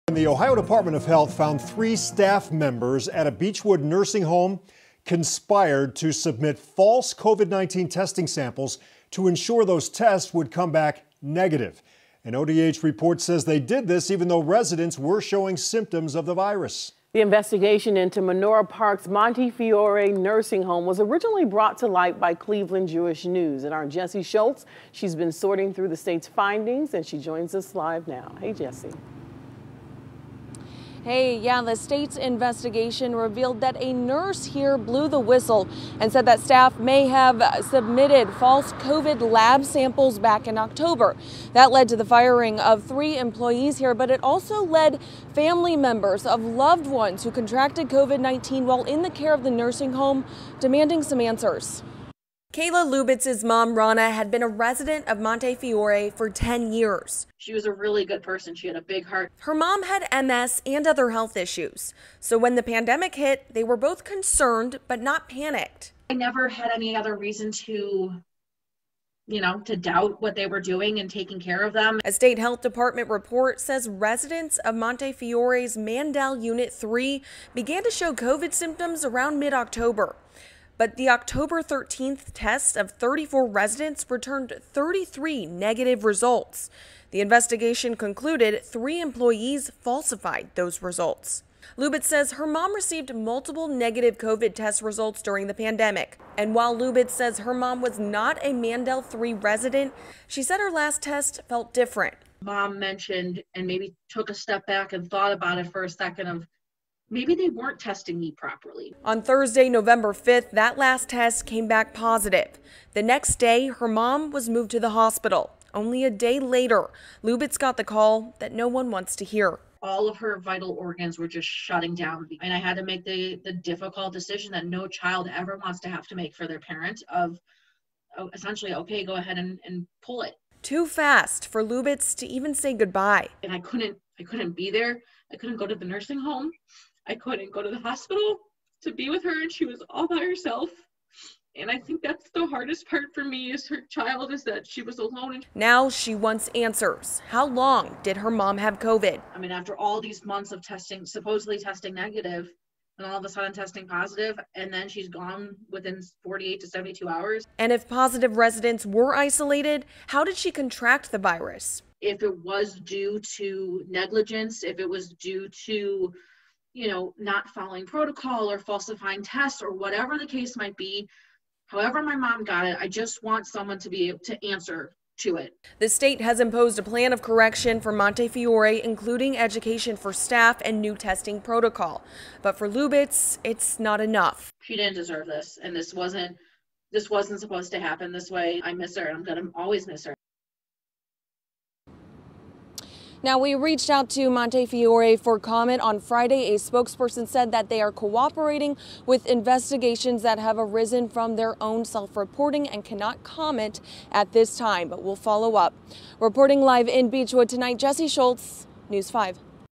State investigation reveals staff at Beachwood nursing home falsified negative COVID lab samples
The Ohio Department of Health’s investigation into COVID-19 protocols at Menorah Park’s Montefiore nursing home found that three staff members conspired to submit false COVID-19 testing samples to ensure that the tests would come back negative.